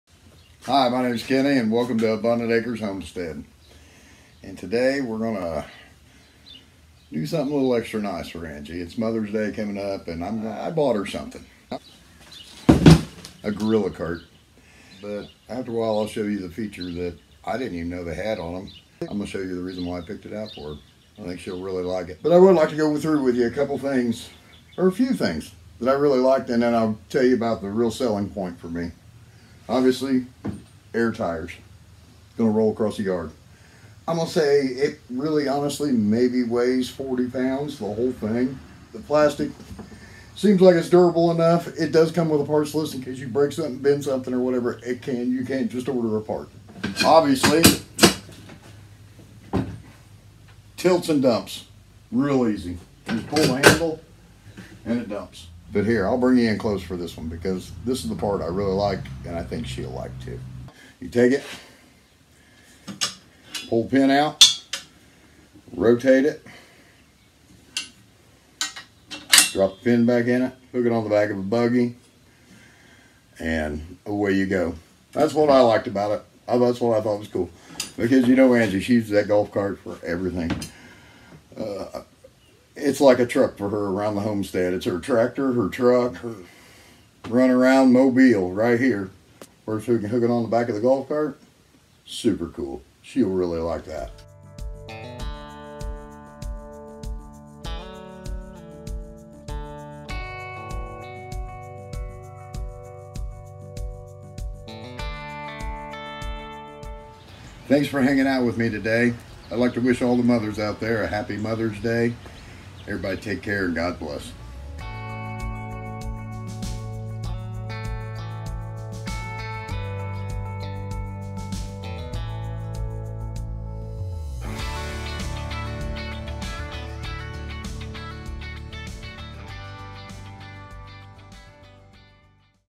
A NEW Tool for the Homestead & A Happy Mother’s Day
It's always a happy day when we get a new tool for the homestead. This one I will DEFINATELY be getting a LOT of use out of! Especially in the garden. Wishing you all a happy and blessed Mother's Day.
Facebook. Abundant Acres
Pinterest. Abundant Acres Homestead

Snail Mail. Abundant Acres
                     Hope, IN 47246